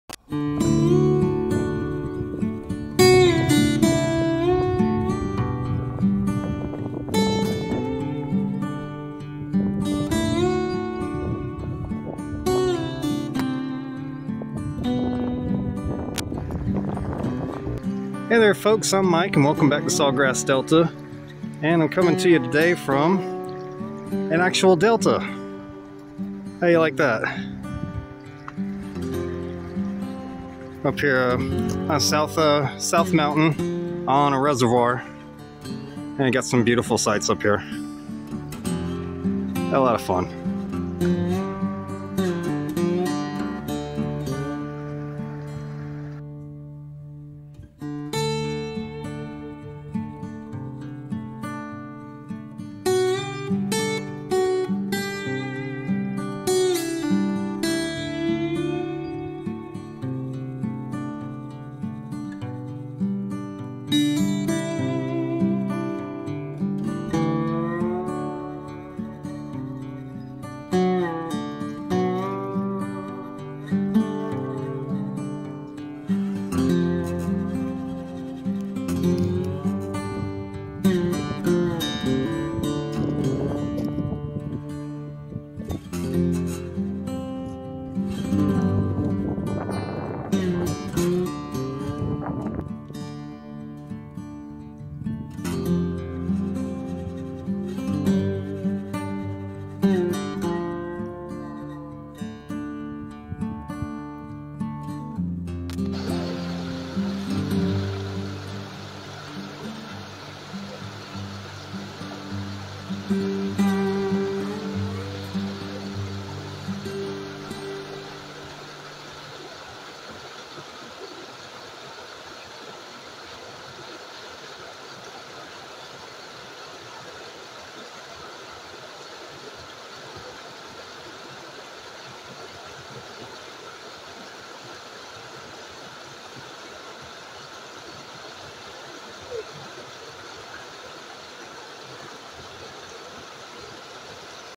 Visiting The Broughton Hospital Reservoir At South Mountain
Today we take a hike around Broughton Hospital Reservoir, a beautiful 19 acre park at South Mountain.